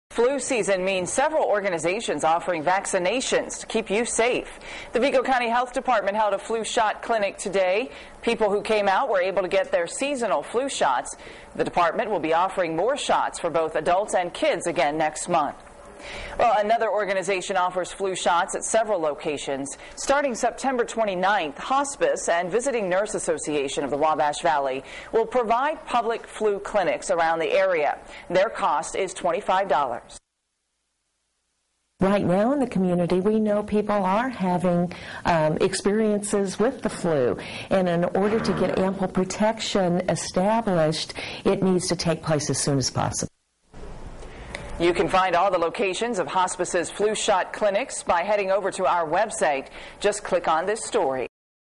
Several flu clinics in Wabash Valley
Flu season means several organizations will be offering vaccinations to keep you safe.